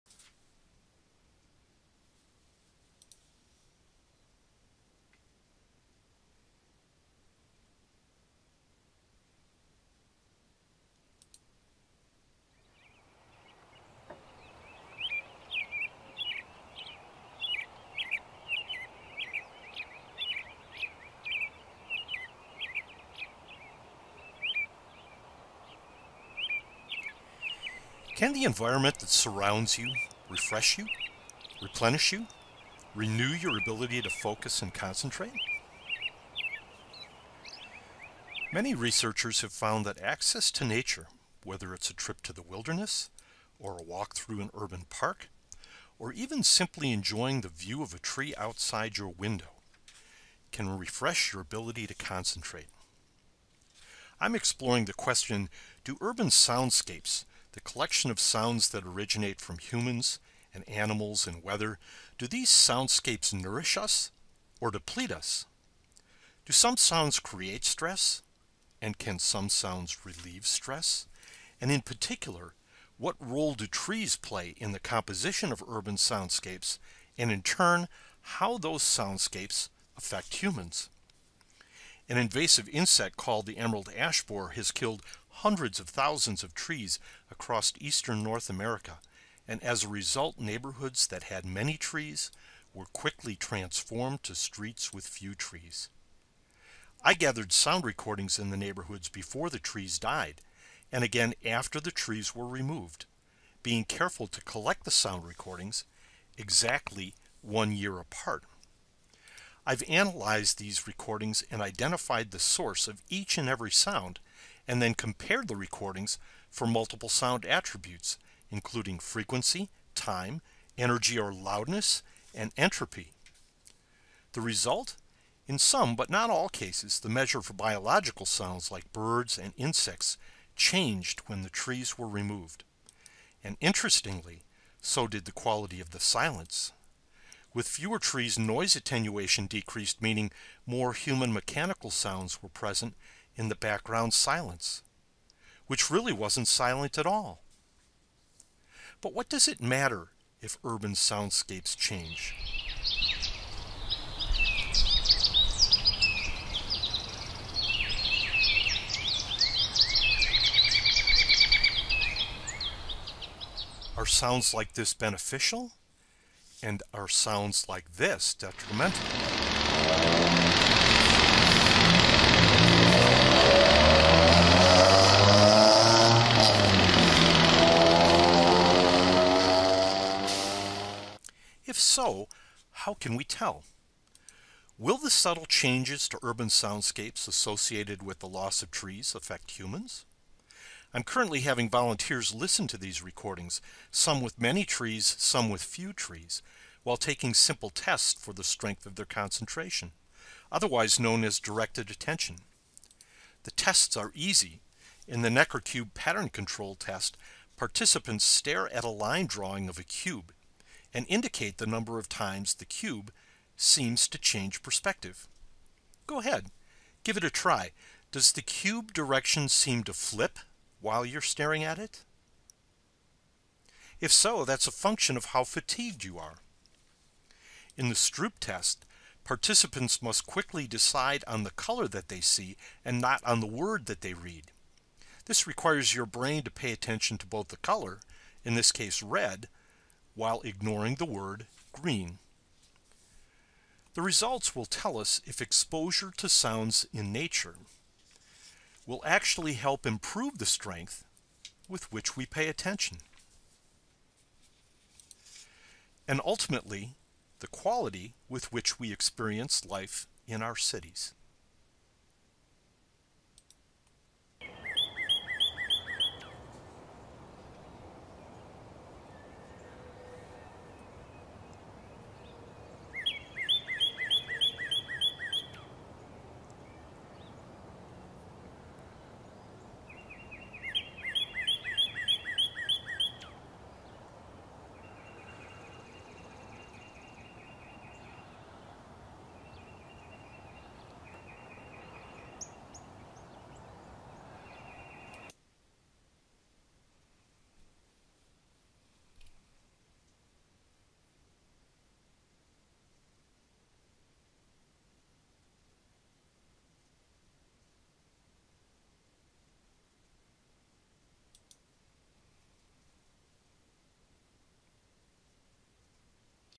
Trees, Sounds, and Human attention
A short video describing research on urban soundscapes and how sounds affect humans.